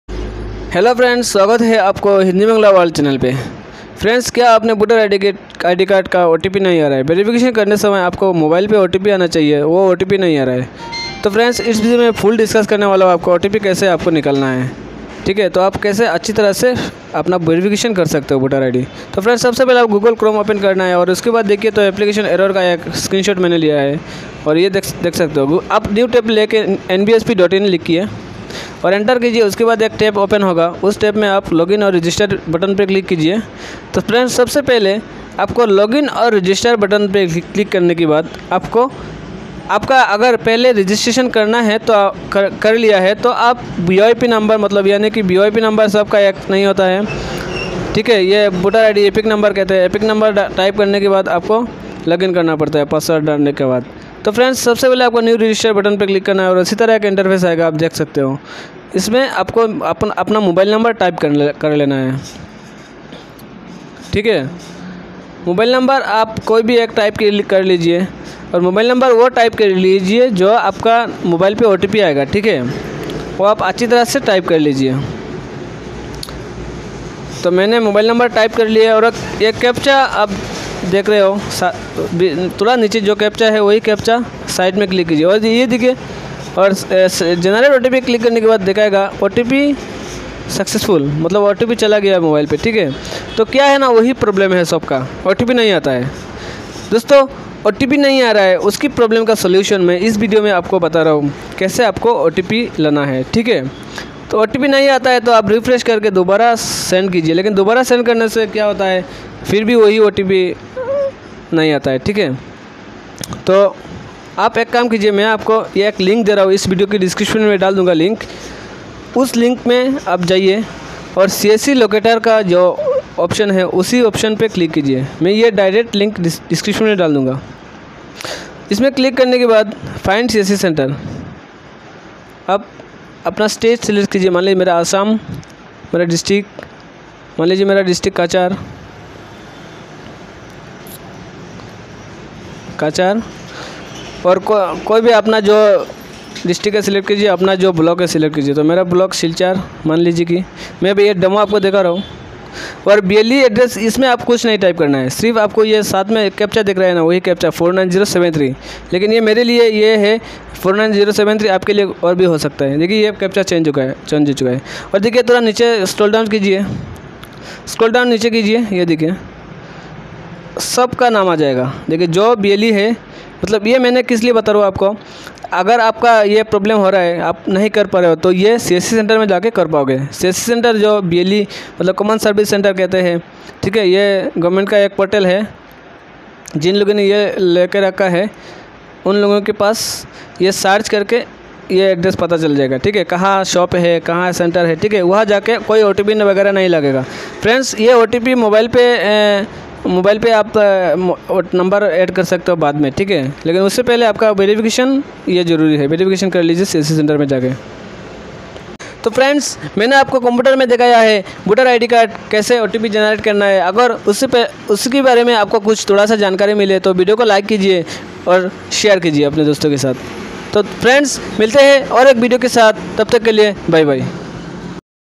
How to Voter id Verification without OTP
Is video pe aapko voter id online verification ki kuch jankari provide kiya asha karta hu aapko pasand aya hoga